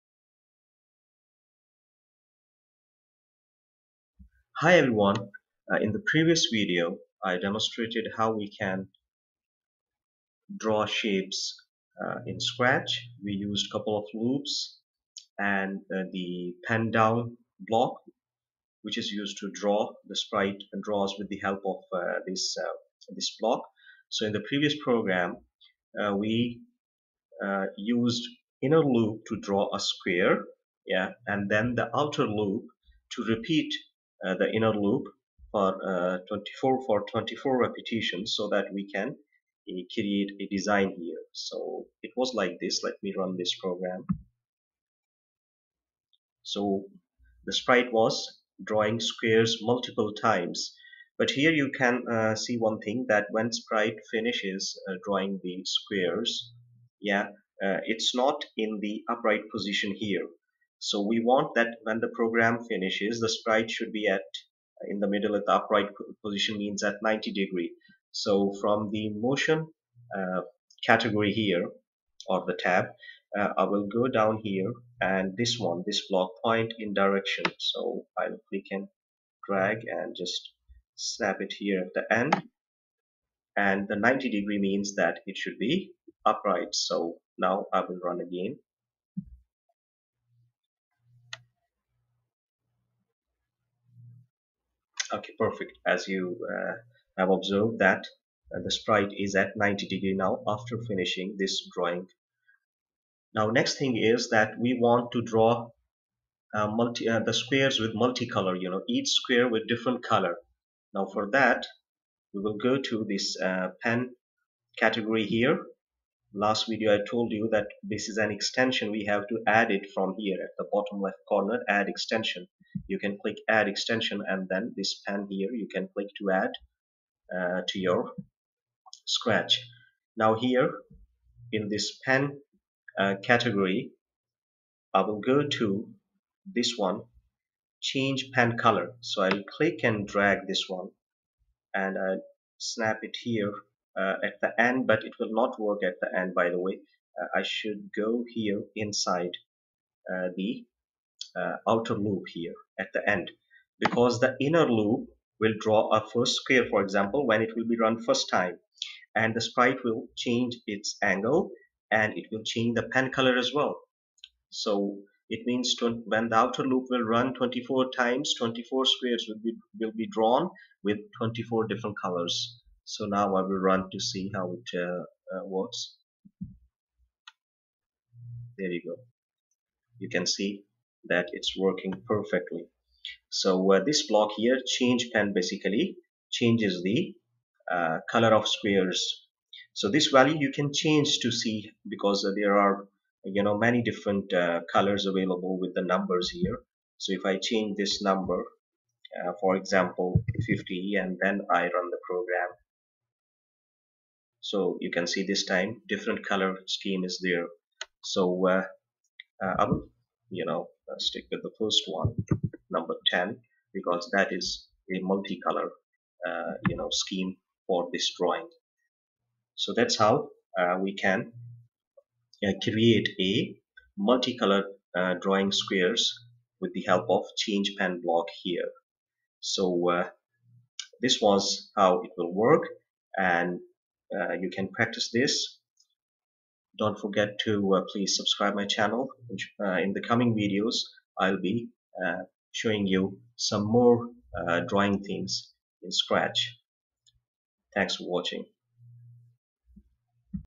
How to Draw Squares Design/Pattern in Scratch | Multi Color
Hi guys! This video is about how to draw squares design/pattern in Scratch. I have made a multi color design using squares. The basic components of any design or pattern in Scratch are loop blocks. Pen category in Scratch has blocks for drawing anything you like!